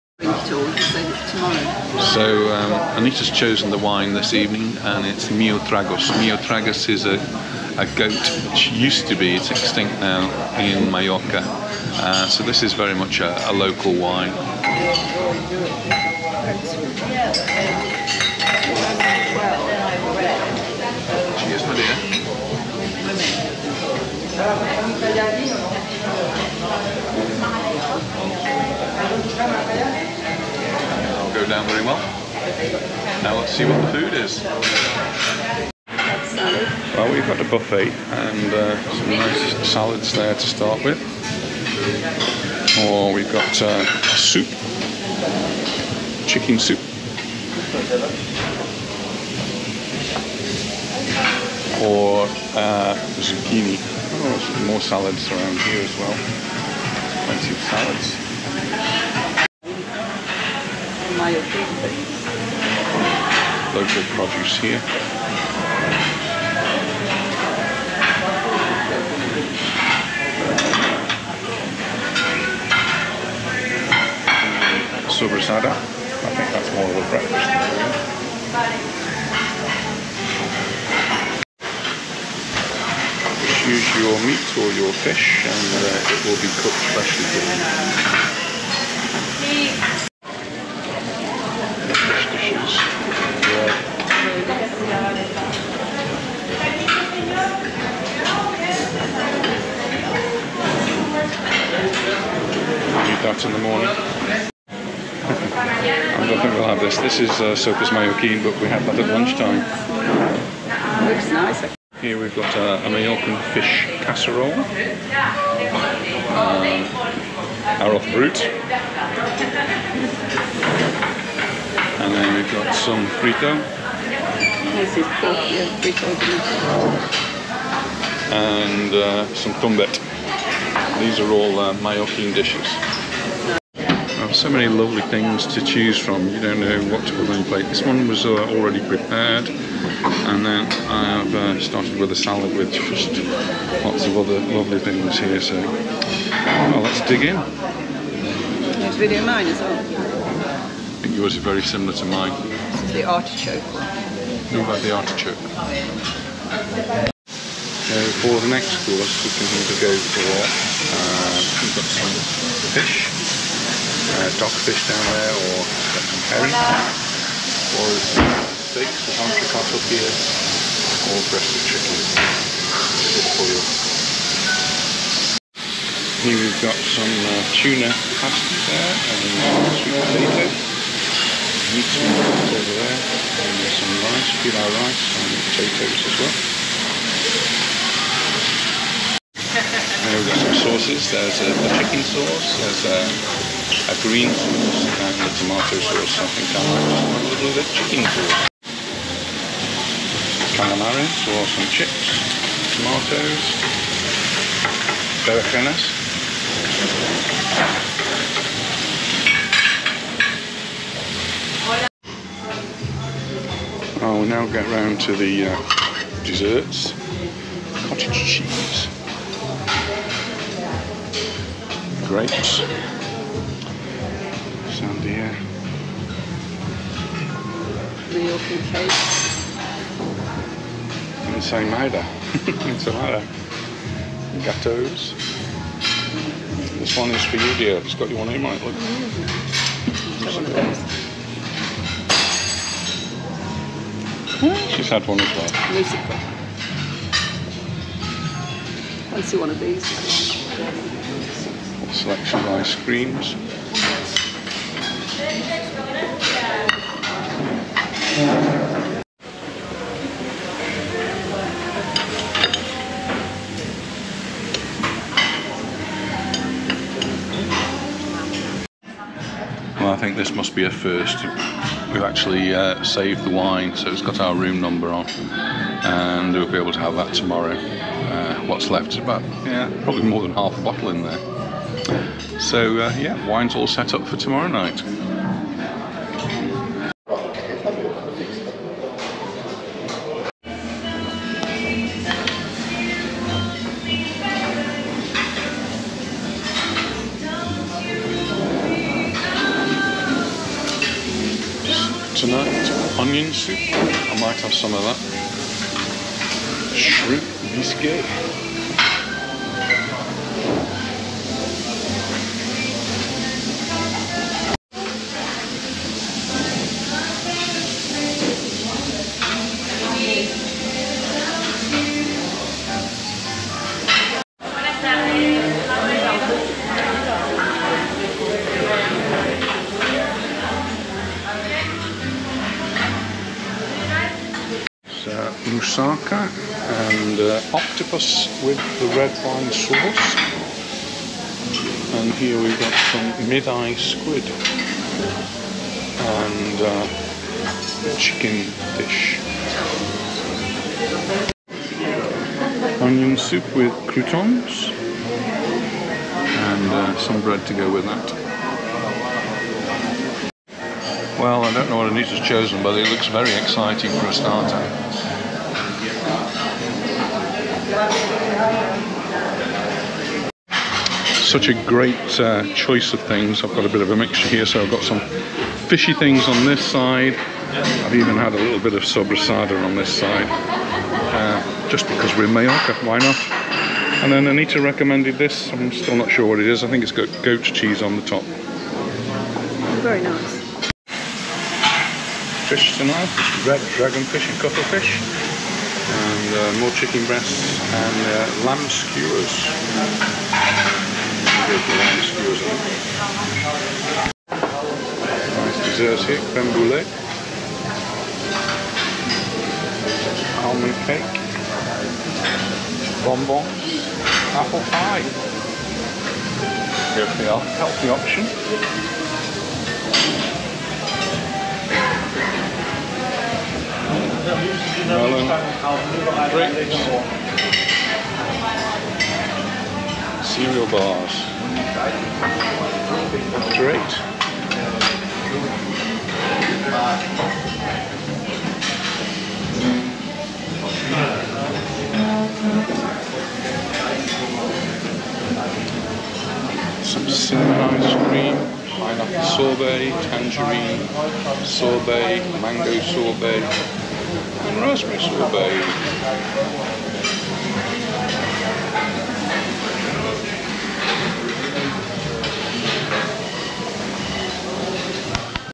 Dinner at the CM Mallorca Palace in Sa Coma - Mallorca - October 2023
Dinner was a self service buffet with lots of choice. Meat and fish were cooked to order and the choice was different each night, with chicken available both nights.
The dining room seemed to be spacious allowing the tables to have plenty of room around them. Seating was inside or out on the terrace. We chose inside, but a lot of people were sitting on the terrace.